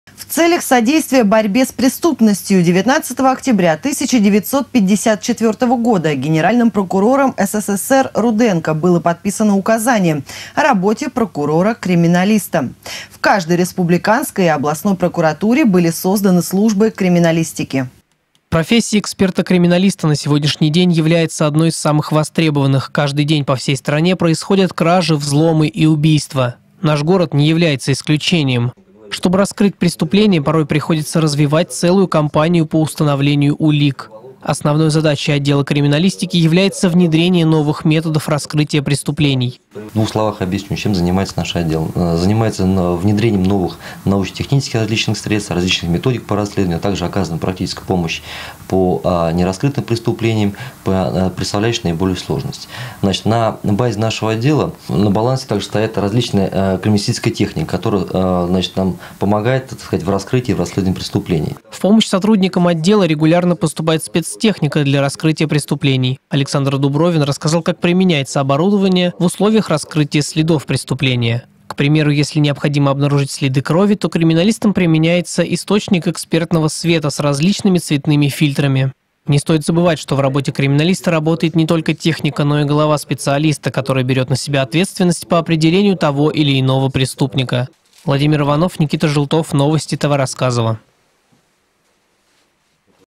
День образования службы криминалистики
В целях содействия борьбе с преступностью 19 октября 1954 года Генеральным прокурором СССР Руденко было подписано Указание «О работе прокурора-криминалиста». В каждой республиканской и областной прокуратуре были созданы службы криминалистики.